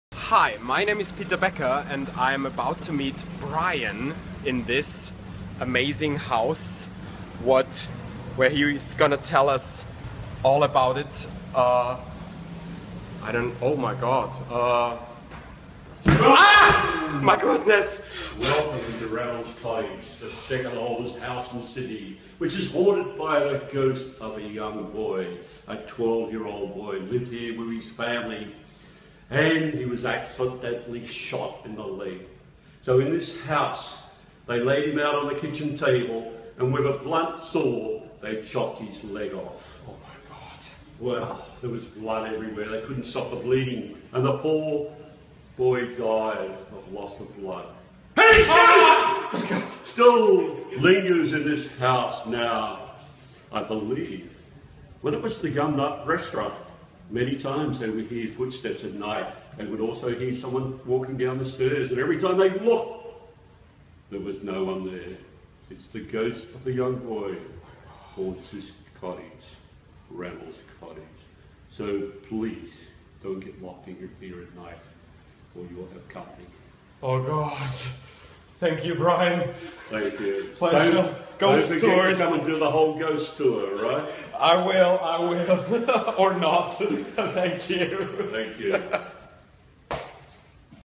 PeterBaeckerLIVE meets Brian from Ghosttours and learns the gruesome secret of Reynolds' cottage
Brian from the Ghosttours tells PeterBaeckerLIVE the gruesome secret of the second oldest house in Sydney: Reynolds' cottage